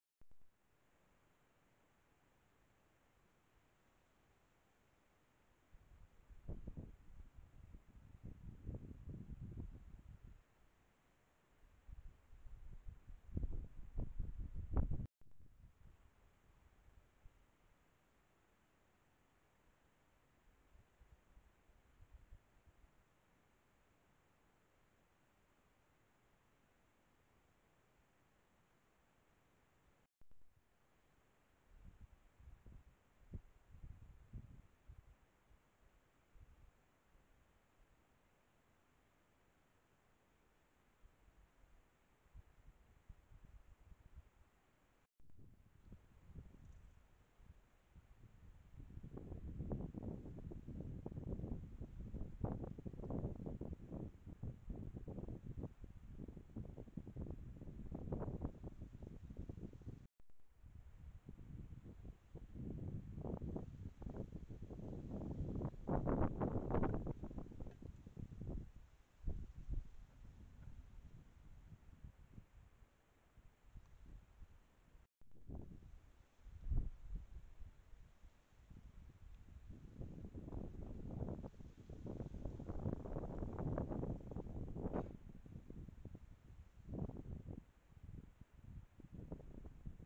27 June 2024 Hedgehog in my garden
my garden camera takes 15sec clips so I have put together the 6 clips captured on the 27th June without editing